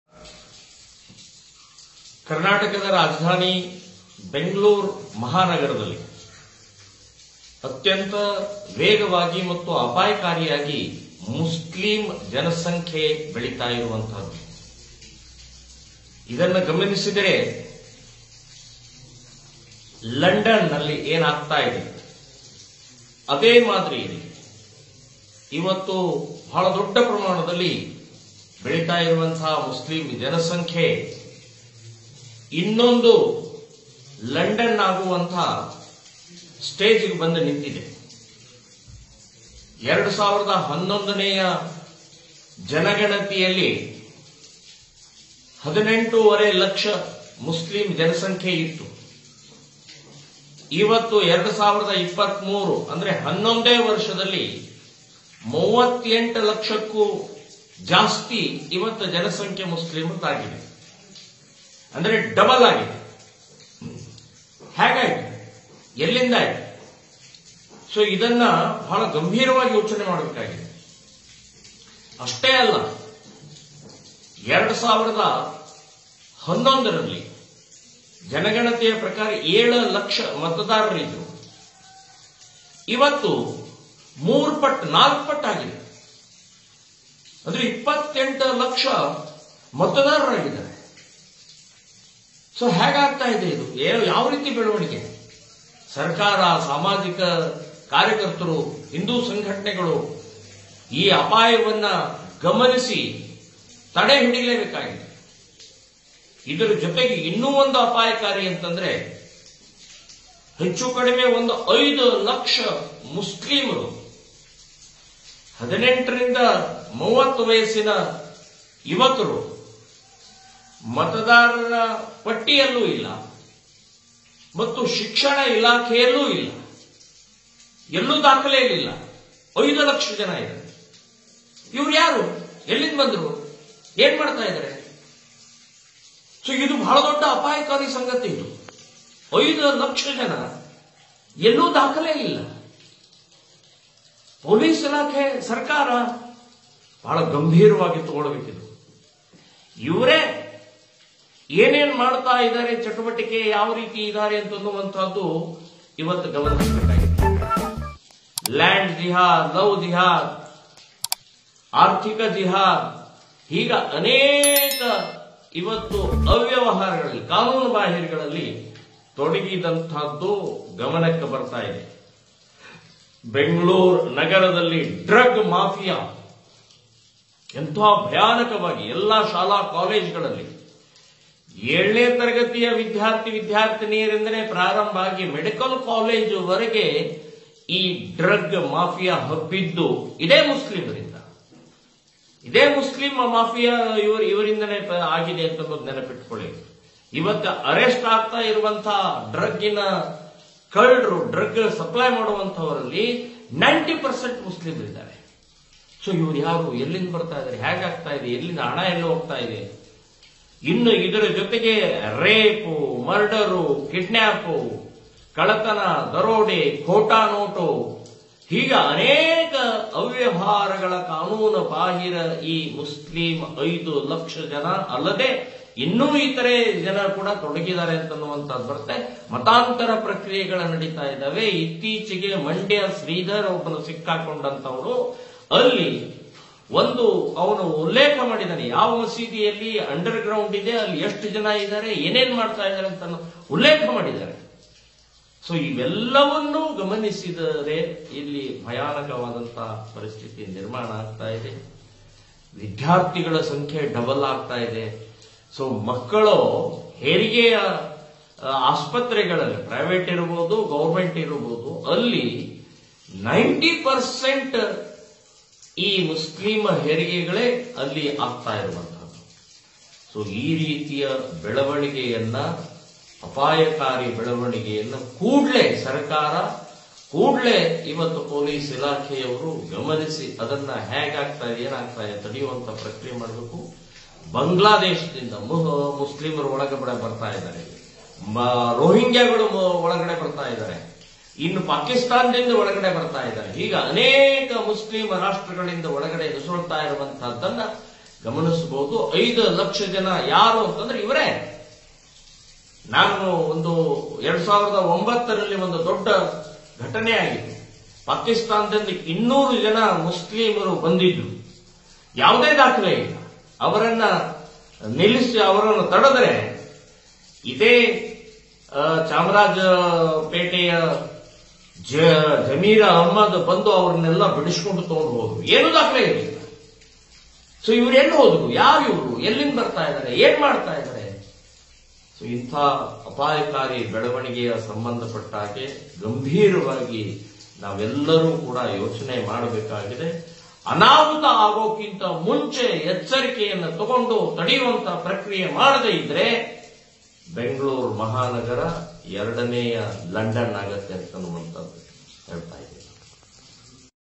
ಬೆಂಗಳೂರಲ್ಲಿ ಮಿತಿಮೀರಿದ ಮುಸ್ಲಿಮರು, ಅಪಾಯ ಗ್ಯಾರಂಟಿ..! | Pramod Muthalik | Muslim Population in Bengaluru
ಬೆಂಗಳೂರಲ್ಲಿ ಮಿತಿಮೀರಿದ ಮುಸ್ಲಿಮರು, ಅಪಾಯ ಗ್ಯಾರಂಟಿ | PramodMuthalik | MuslimPopulation in Bengaluru